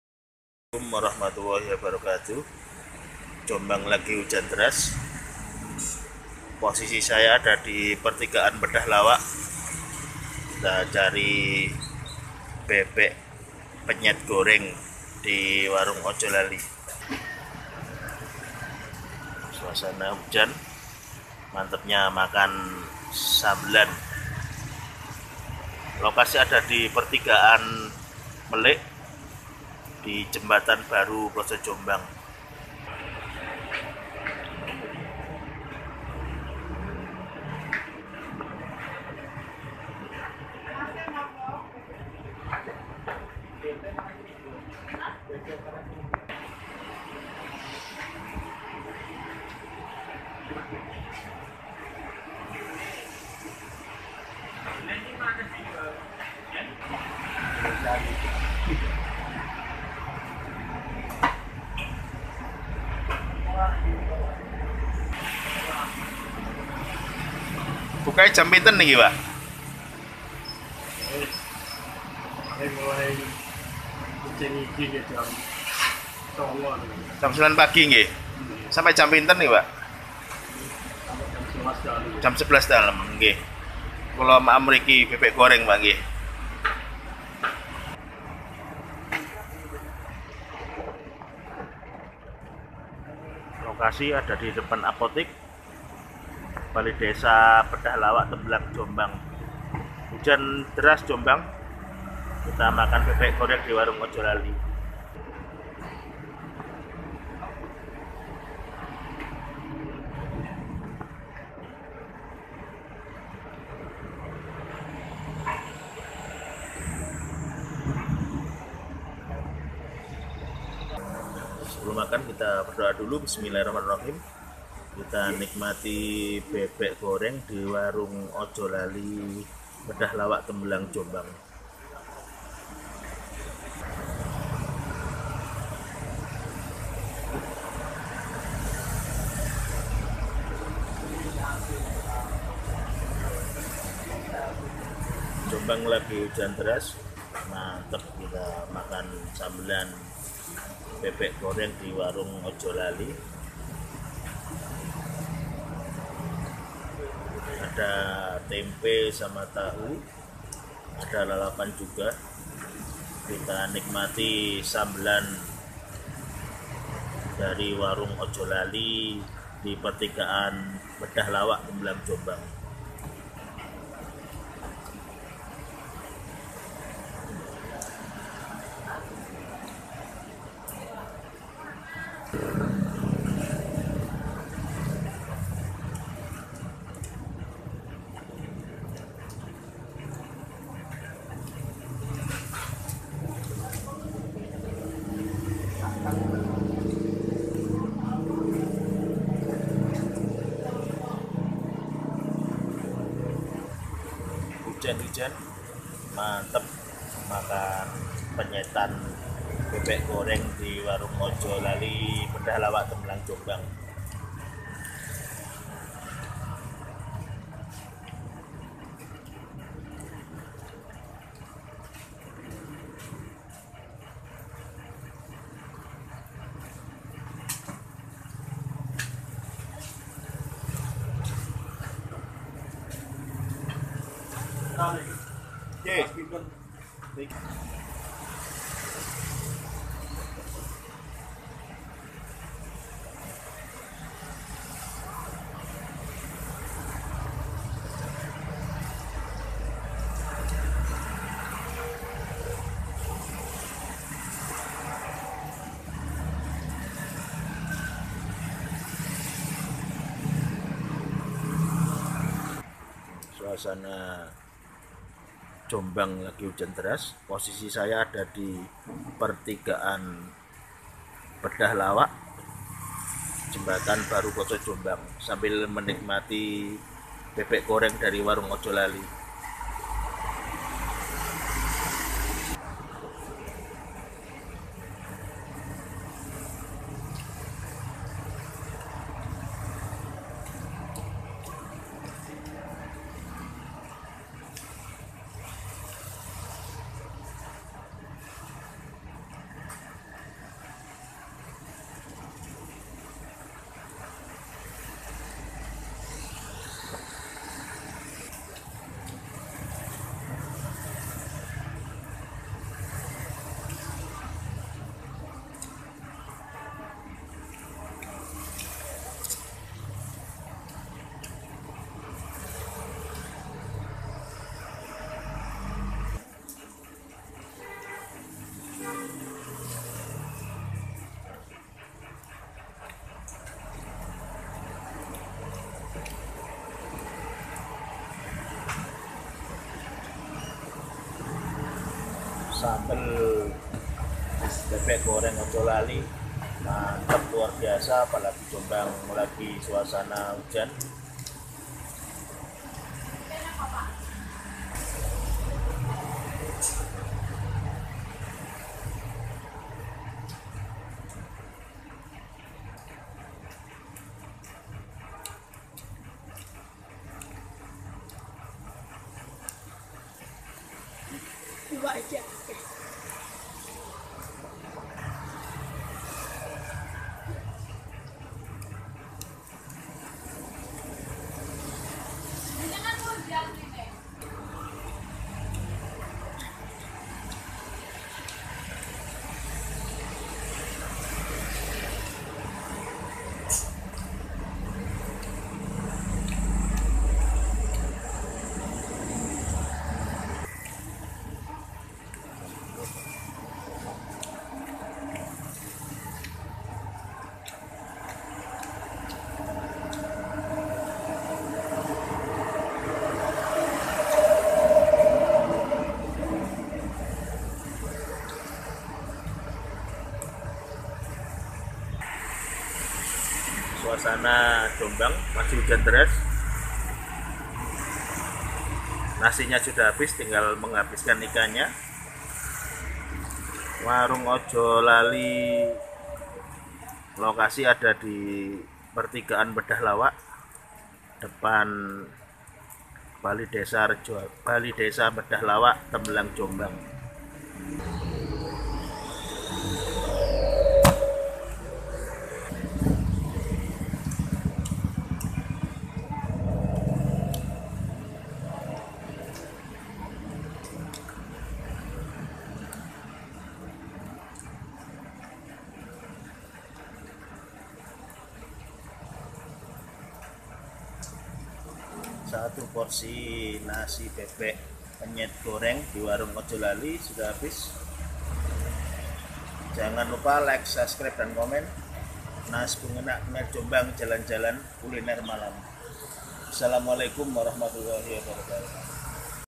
SAMBELNYA MELIMPAH,PENYETAN LAMONGAN.IDE USAHA MODAL KECIL
kuliner jombang kali ini menyajikan penyetan ayam,lokasi pertigaan bedah lawak tembelang jombang.
mbak suci iwak kali